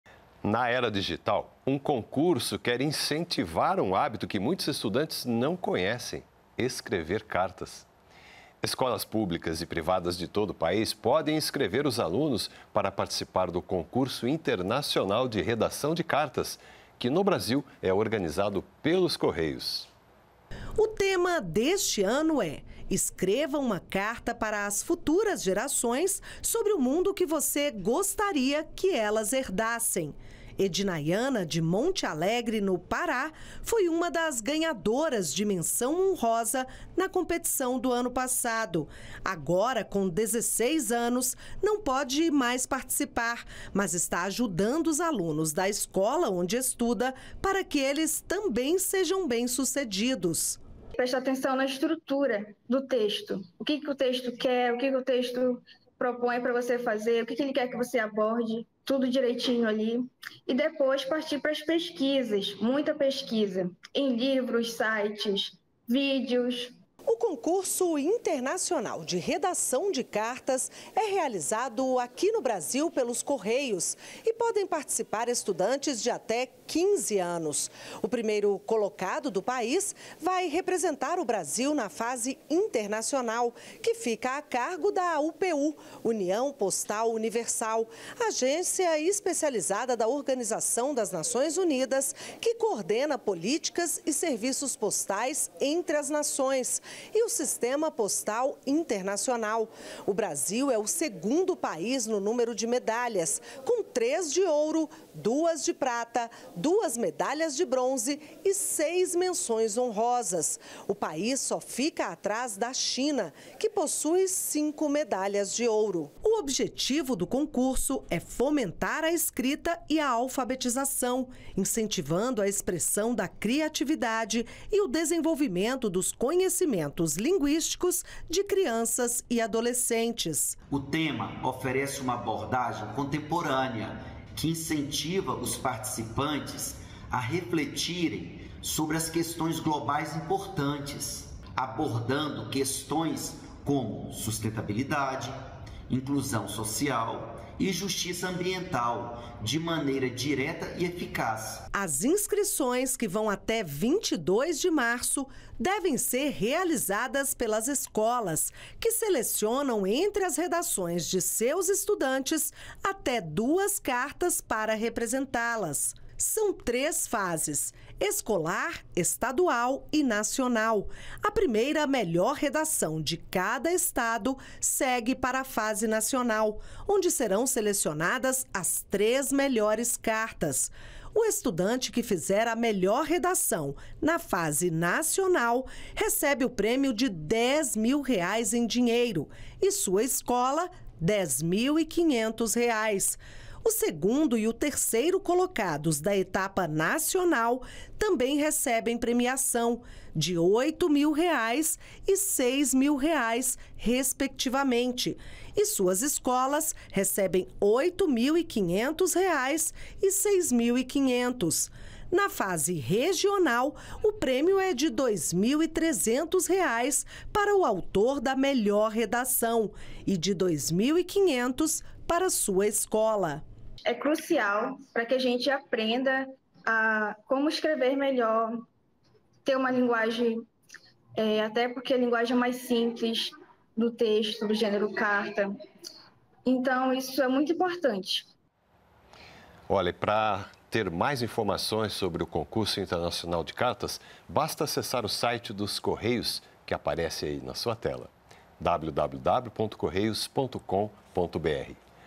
CORREIOS ESTÃO COM INSCRIÇÕES ABERTAS PARA CONCURSO INTERNACIONAL DE REDAÇÃO DE CARTAS
Os Correios estão com inscrições abertas para o Concurso Internacional de Redação de Cartas. O tema deste ano é: “escreva uma carta para as futuras gerações sobre o mundo que você gostaria que elas herdassem”. O objetivo do concurso é incentivar esse hábito que muitos jovens não conhecem.